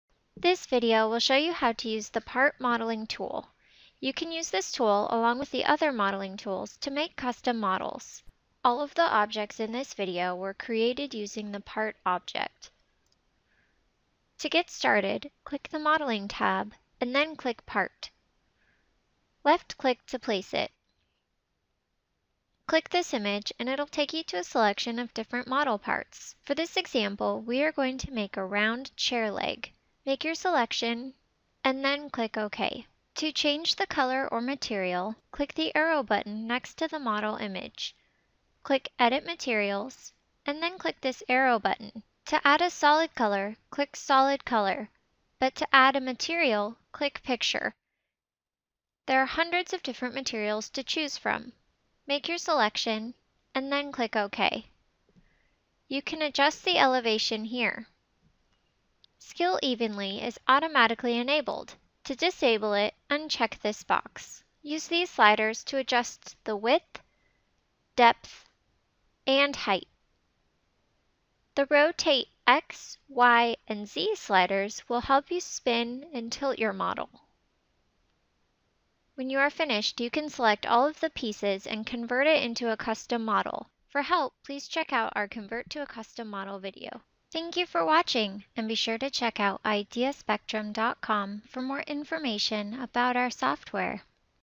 Realtime Landscaping- Part
This video will show you how to use the part modeling tool. You can use this tool along with the other modeling tools to make custom models.